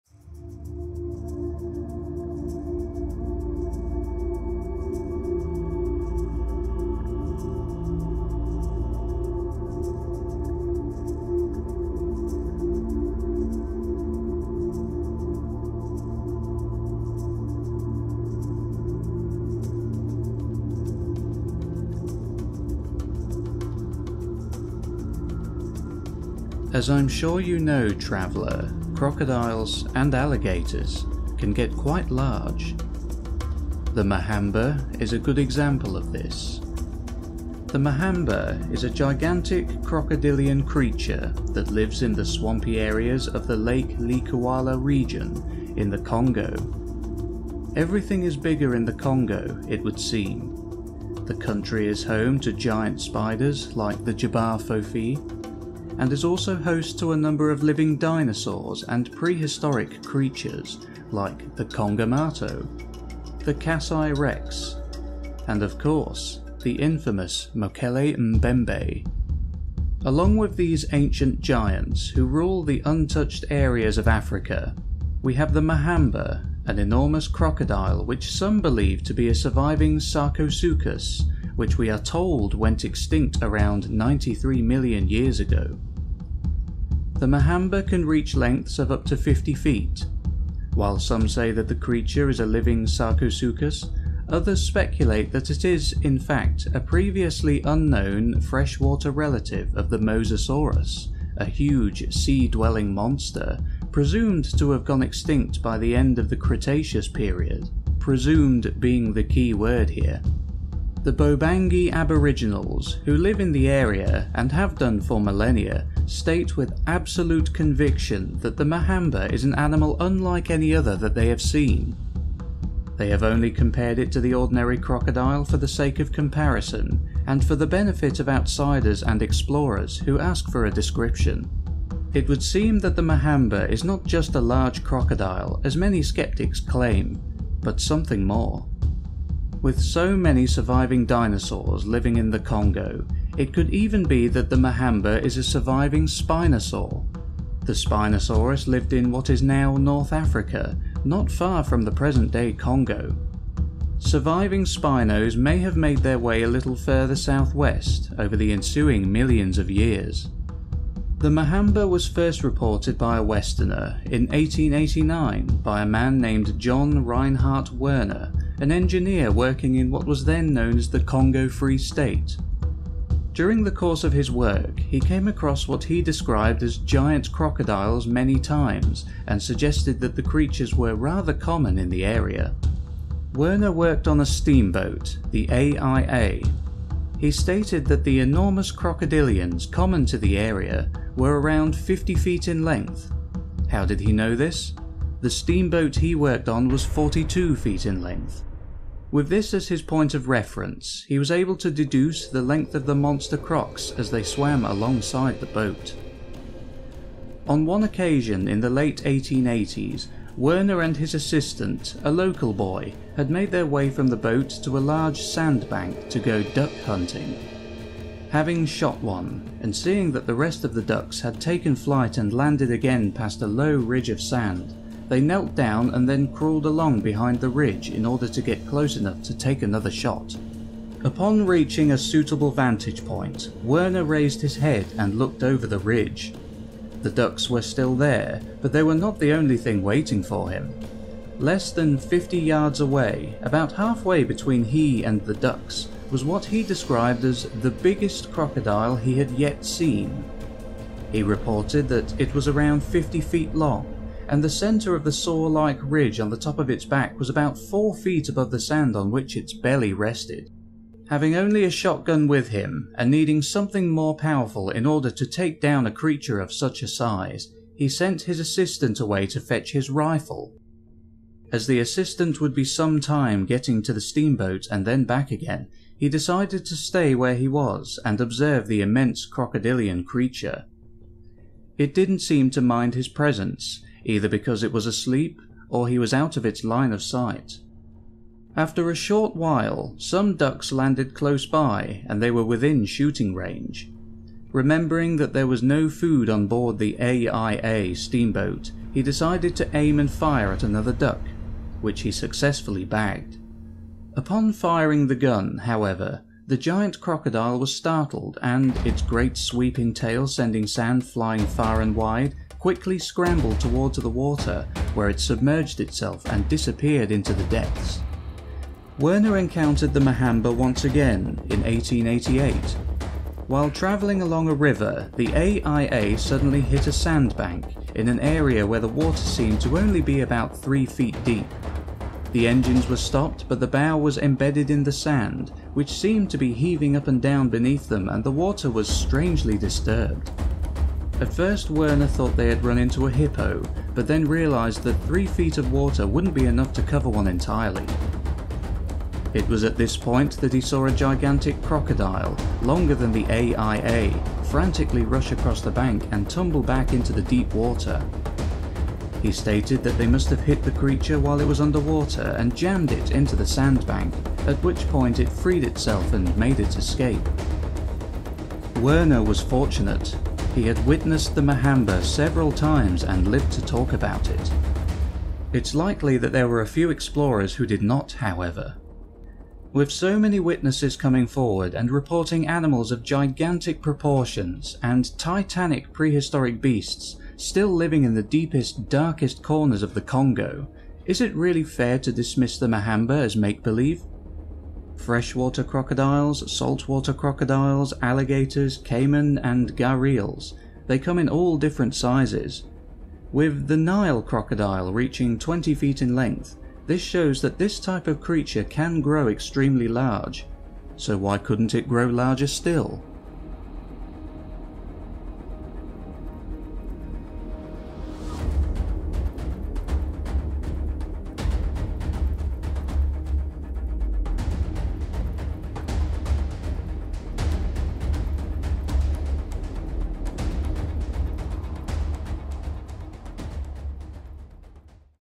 Mahamba - A Short Documentary
A brand new and exclusive short documentary on the Mahamba.

As I'm sure you know, traveller, crocodiles and alligators can get quite large.

The Mahamba is a good example of this.

The Mahamba is a gigantic crocodilian creature, that lives in the swampy areas of the Lake Likouala region, in the Congo... Everything is bigger in the Congo, it would seem. The country is home to giant spiders, like the J'ba FoFi... and is also host to a number of living dinosaurs and prehistoric creatures, like the Kongamato... the Kasai Rex... and of course, the infamous Mokele-mbembe...

Along with these ancient giants who rule the untouched areas of Africa, we have the Mahamba, an enormous crocodile which some believe to be a surviving sarcosuchus, which we are told went extinct around 93 million years ago.

The Mahamba can reach lengths of up to 50 feet. While some say that the creature is a living sarcosuchus, others speculate that it is, in fact, a previously unknown, freshwater relative of the mosasaurus, a huge, sea-dwelling monster, presumed to have gone extinct by the end of the Cretaceous period... presumed, being the key word here.

The Bobangi aboriginals, who live in the area and have have done for millennia, state with absolute conviction that the Mahamba is an animal unlike any other they have seen... they have only compared it to the ordinary crocodile for the sake of comparison, and for the benefit of the outsiders and explorers who ask for a description. It would seem that the Mahamba is not just a large crocodile, as many sceptics claim, but something more...

With so many surviving dinosaurs living in the Congo, it could even be that the Mahamba is a surviving Spinosaur. The Spinosaurus lived in what is now North Africa, not far from the present day Congo. Surviving Spinos may have made their way a little further Southwest over the ensuing millions of years.